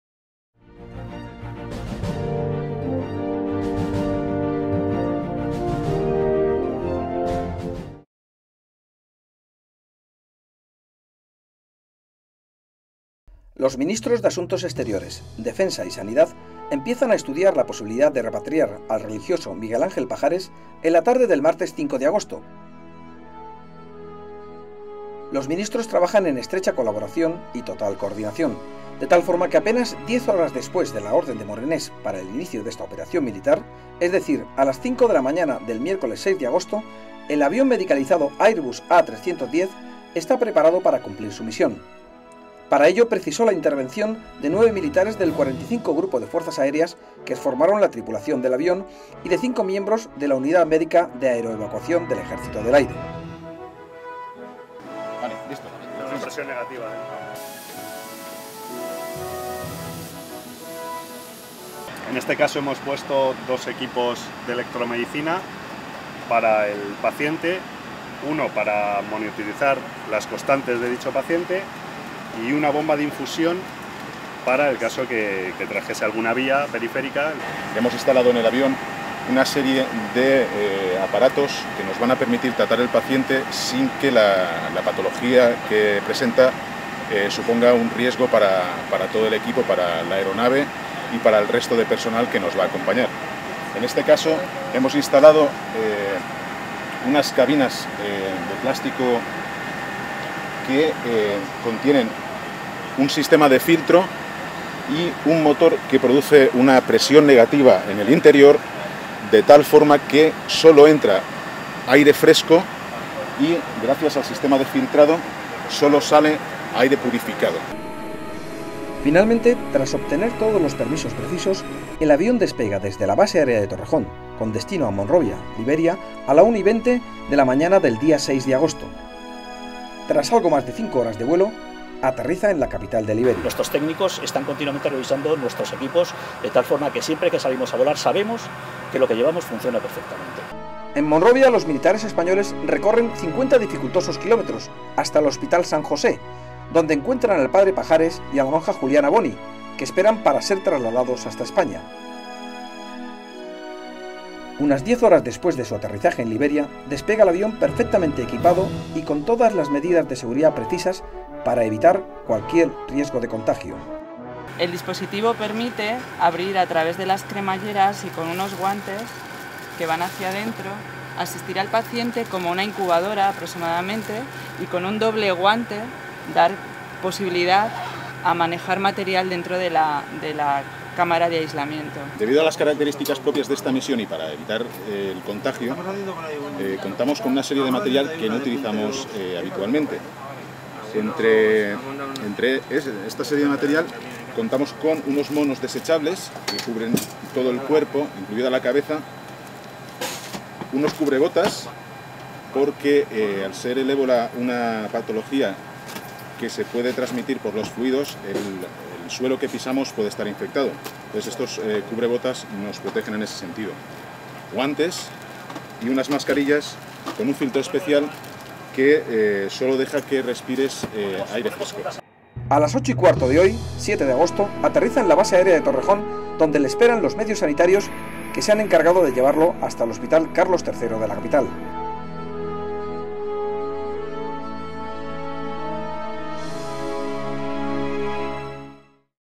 Defensa repatría al religioso Miguel Pajares
Los ministros de Asuntos Exteriores, Defensa y Sanidad empiezan a estudiar la posibilidad de repatriar al religioso Miguel Ángel Pajares en la tarde del martes 5 de agosto.

Los ministros trabajan en estrecha colaboración y total coordinación, de tal forma que apenas 10 horas después de la orden de Morenés para el inicio de esta operación militar, es decir, a las 5 de la mañana del miércoles 6 de agosto, el avión medicalizado Airbus A310 está preparado para cumplir su misión. Para ello precisó la intervención de 9 militares del 45 Grupo de Fuerzas Aéreas que formaron la tripulación del avión y de 5 miembros de la Unidad Médica de Aeroevacuación del Ejército del Aire.

Finalmente, tras obtener todos los permisos precisos, el avión despega desde la base aérea de Torrejón con destino a Monrovia (Liberia) a la 01.20 horas de la mañana del día 6 de agosto. Tras algo más de 5 horas de vuelo, aterriza en la capital de Liberia.
En Monrovia, los militares españoles recorren 50 dificultosos kilómetros hasta el hospital San José, donde encuentra al padre Pajares y a la monja Juliana Bohni, que esperan para ser trasladados a España.

Unas 10 horas después de su aterrizaje en Liberia despega el avión, perfectamente equipado y con todas las medidas de seguridad precisas para evitar cualquier riesgo de contagio.
A las 08.15 horas del 7 de agosto, aterrizó en la base aérea de Torrejón, donde le esperaban los medios sanitarios, que se encargaron de llevarlo hasta el Hospital Carlos III de Madrid.